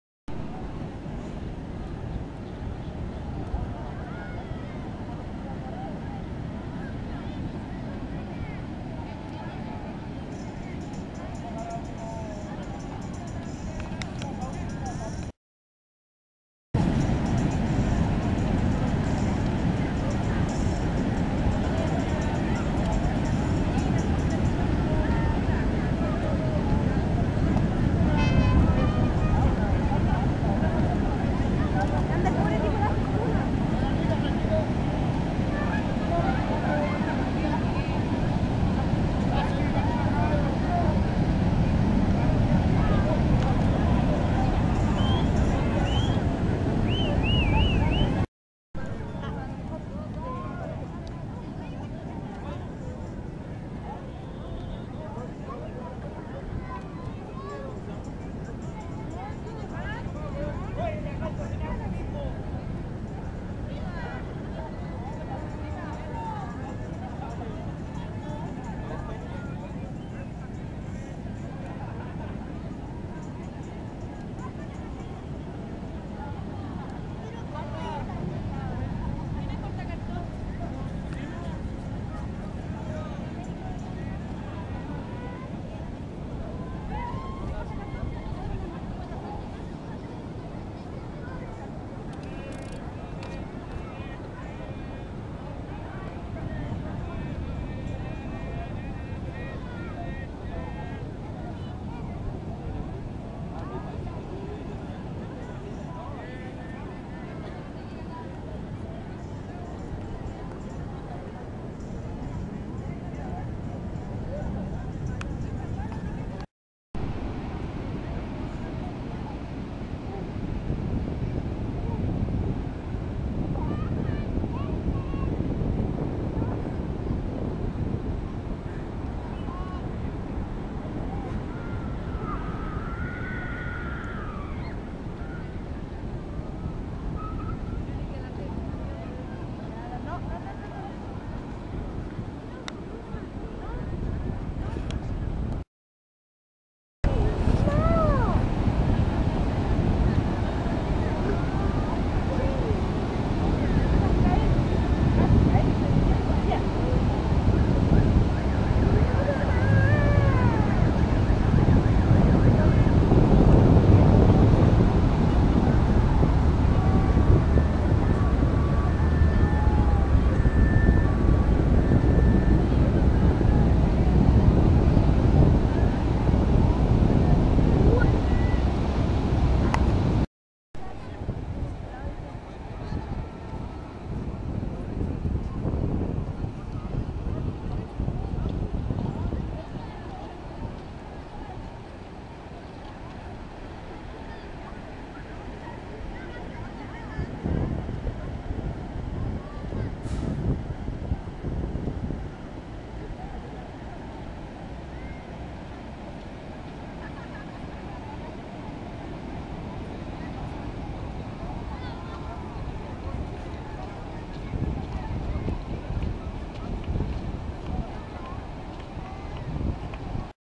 Cartagena de Chile Velaton en La Playa Chica Carnaval por la Tierra 2010
Grupo Inquilinos Intervenciones invito a toda la comunidad a una gran velaton que se realizo en la Playa Chica, esto para finalizar el Carnaval por la Tierra 2010. La intencion de inquilinos es crear actividades que integren a la comunidad, que esta NO SEA UN MERO ESPECTADOR, SI NO, UN PARTICIPANTE ACTIVO DEL TRABAJO.
titulo: EN PRIMERA PERSONA, DESDE LAS 19 HORAS ESTUVIERON EN LA PLAYA, CADA PARTICIPANTE LLEVO UNA VELA MAS UN CARTUCHO DE PAPEL O UNA BOTELLA PLASTICA PARTIDA POR LA MITAD. ES IMPORTANTE DESTACAR QUE SE LIMPIO LA PLAYA DE LOS RESIDUOS QUE SE UTILIZARON .
Video @ Edicion
Alejandro Correa Ortiz